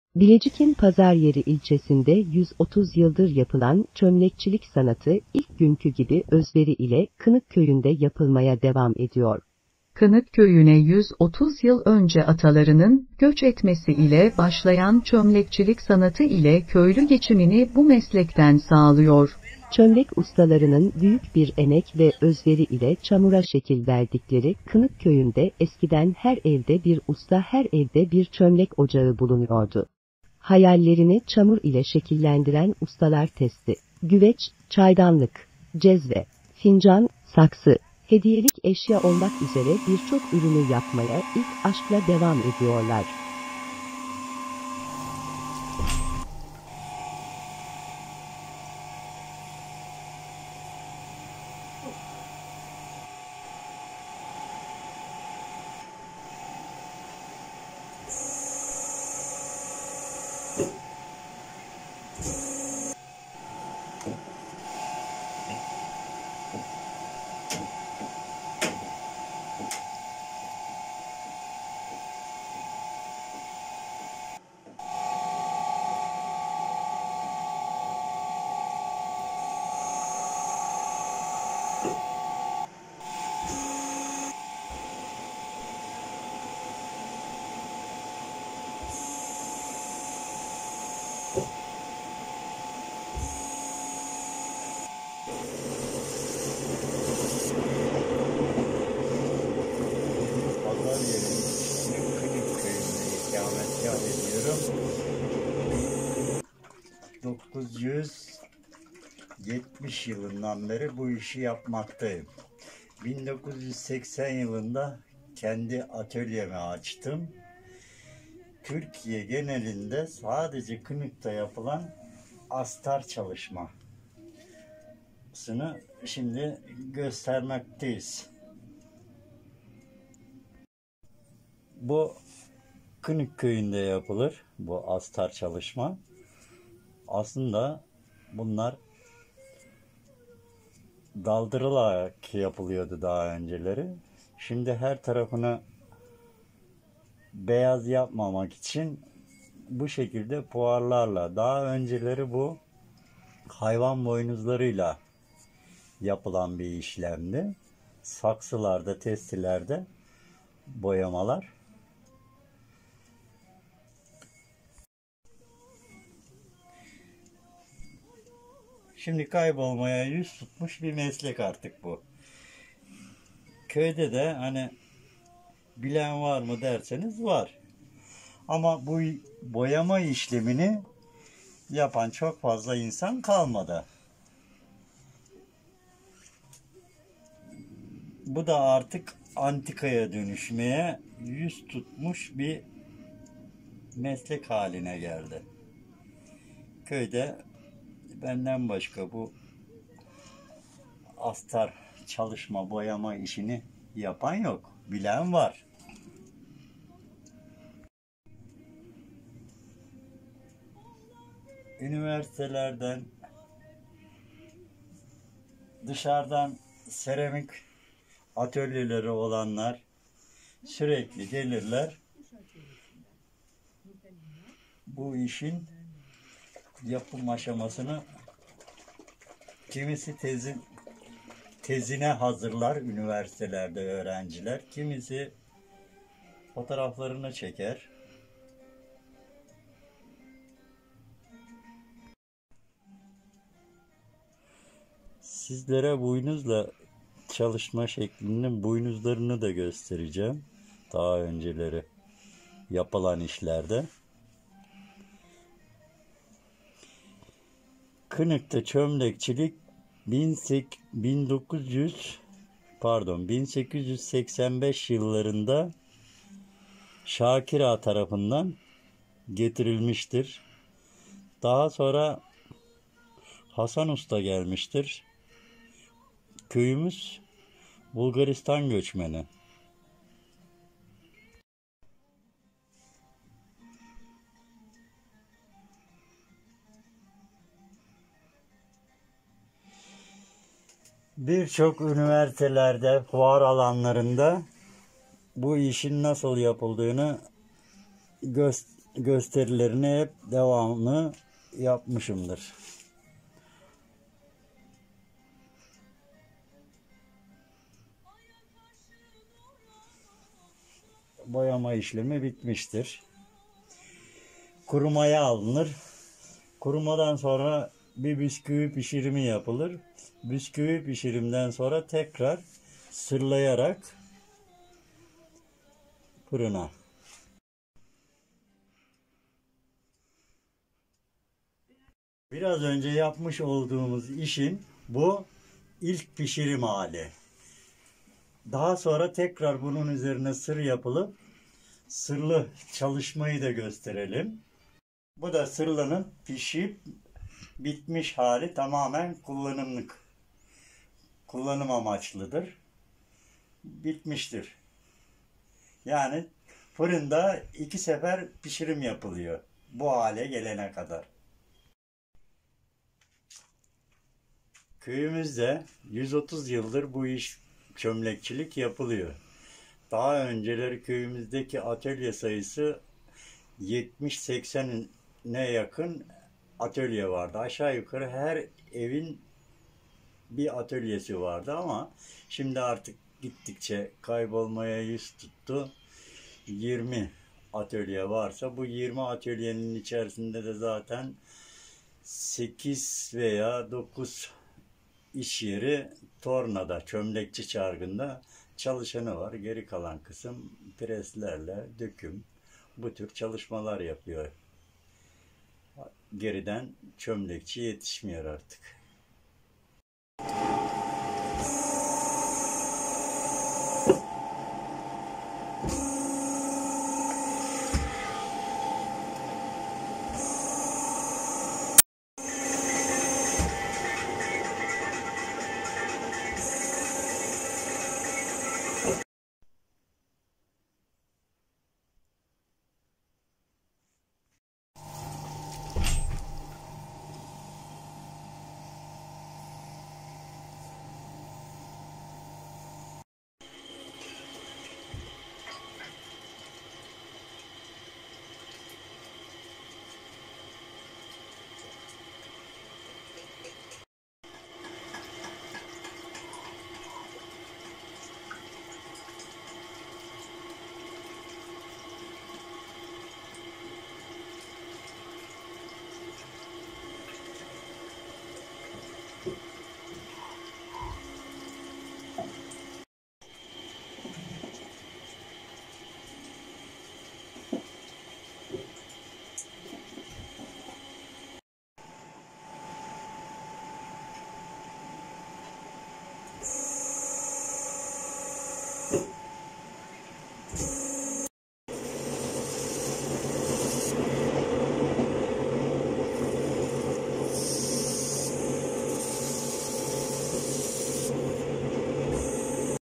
Balkanlardan gelen atalarının mirasını hala sürdürüyorlar
Bilecik'in Pazaryeri'ne ilçesine uzun yıllar önce Balkanlardan göç edenlerle birlikte yapımına başlanılan çömlekçilik sanatı hala sürdürülürken, pres veya döküm dışında eski usulle çarkla üretim yapanların sayısı hayli azaldı.

İlçenin Kınık Köyü'ne 130 yıl önce atalarının göç etmesi ile başlayan çömlekçi sanatı ile uğraşan köylülerin çoğunluğu geçimini topraktan sağlıyor. Çömlek ustalarının büyük bir emek ve özveri ile çamura şekil verdikleri Kınık köyünde eskiden her evde bir usta, her evde bir çömlek ocağı bulunuyordu. Son zamanlarda köydeki gençlerin büyük şehirlere göç etmesiyle çömlek ocaklarının sayıları azalsa da Kınık köyünde çömlek çarkları hala sanat için dönüyor. Hayallerini çamur ile şekillendiren ustalar testi, güveç, çaydanlık, cezve, fincan, saksı, hediyelik eşya olmak üzere birçok ürünü yapmaya ilk aşkla devam ediyorlar.

"Şuan 20 atölye varsa bunların 8 veya 9 tanesi tornada çömlekçi çarkında üretim yapıyor"

53 yıldır çömlek ustalığı yapan Osman Menteş, köylerinde eskisi gibi çömlek işiyle uğraşan kalmadığı söyleyerek, "Kınık köyümüzde 130 yıldır bu çömlekçilik işi yapılıyor. Eskiden köyümüzdeki atölye sayısı 70-80 adetti. Aşağı yukarı köyde her evin bir atölyesi vardı. Şimdi artık bu meslek kaybolmaya yüz tuttu. Şuan 20 atölye varsa bunların da 8 veya 9 tanesi tornada çömlekçi çarkında üretim yapıyor. Geri kalan kısım pres veya dökümde yapıyor bu işi. Geriden çömlekçi yetişmiyor artık" dedi.